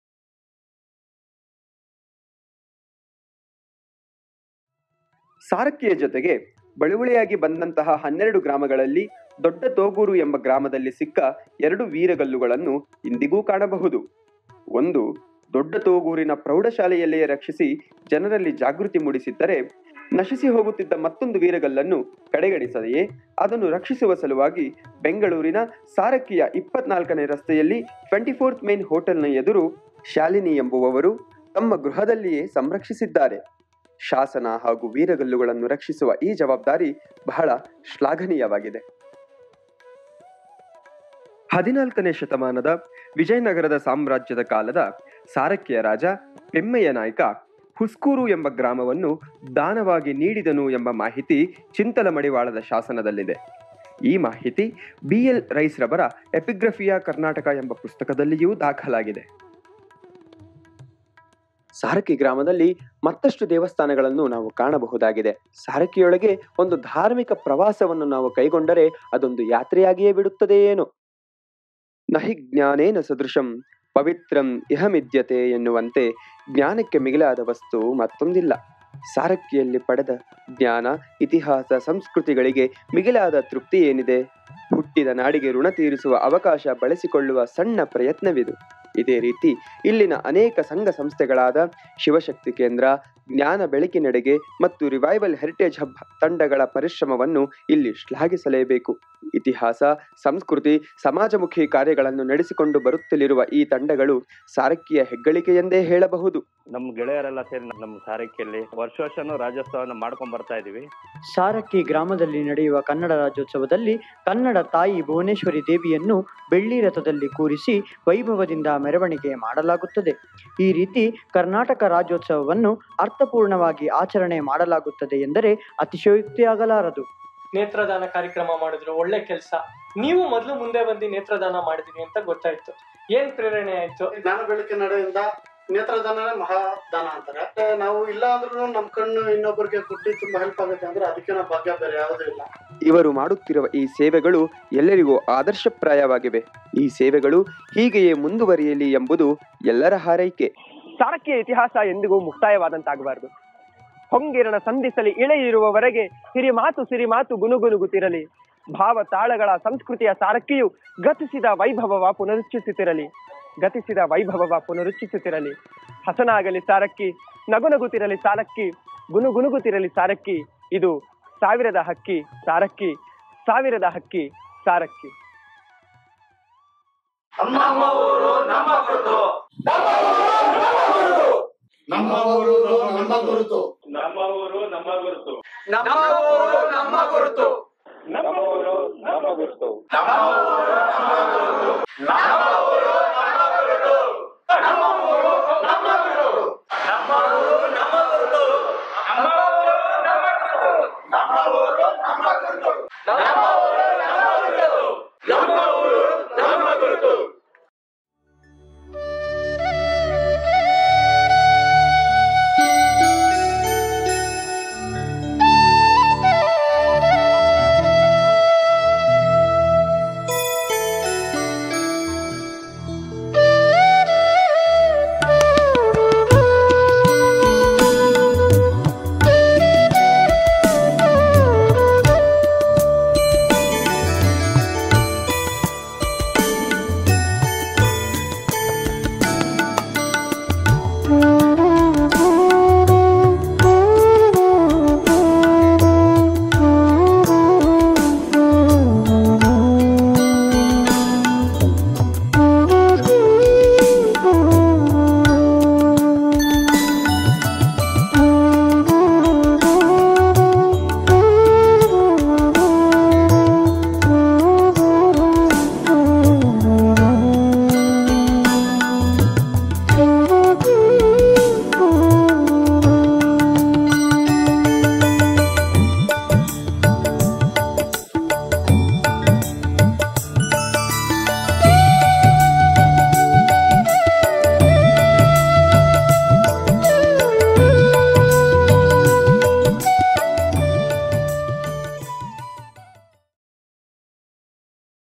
Sarakki Documentary Part 6:6
Part 6/6
ಸಾರಕ್ಕಿಯ ಶಾಸನಗಳು ಮತ್ತು ಯುವಕರ ಸಾಮಾಜಿಕ ಕೊಡುಗೆಗಳು.
Sarakki's inscriptions and Youths contribution.

ಸಾರಂಗಧರನ ಸಾರಕ್ಕಿ,
ನಮ್ಮ ಊರು ನಮ್ಮ ಗುರುತು

ಸಾರಕ್ಕಿ ಎಂಬ ಐತಿಹಾಸಿಕ ಗ್ರಾಮದ ಅಪೂರ್ವ ಹಾಗೂ ಅಮೂರ್ತ ವಿಷಯಗಳ ಅನಾವರಣವೇ ಈ ಸಾಕ್ಷ್ಯಚಿತ್ರ. ಗ್ರಾಮದ ಇತಿಹಾಸ, ಅಭೂತಪೂರ್ವ ಎನ್ನುವ ವಿಷಯಗಳು ಇದರಲ್ಲಿ ಅಡಕವಾಗಿದೆ. ಇತಿಹಾಸದ ರಾಜ ಮಹಾರಾಜರು, ಅಂದಿನ ಹಾಗೂ ಇಂದಿನ ಜೀವನಕ್ಕೆ ಪೂರಕವಾಗಿ ಆಧ್ಯಾತ್ಮ, ಕಲೆ, ಸಾಹಿತ್ಯ, ಜೀವನ ಶೈಲಿ ಹಾಗೂ ಹಿಂದಿನ - ಇಂದಿನ ಜೀವನ ಆಗುಹೋಗುಗಳ ಚಿತ್ರಣವೇ ಇಲ್ಲಿನ ಹೆಗ್ಗುರುತು.

Sarangadharana Sarakki,
Our hometown is our identity
The documentary is the unveiling of the unique and intriguing themes of the historic village of Sarakki. The history of the village, including the unprecedented. The landmark is the portrayal of the great Maharajas of history, mysticism, art, literature, life style of past - present life, then and now.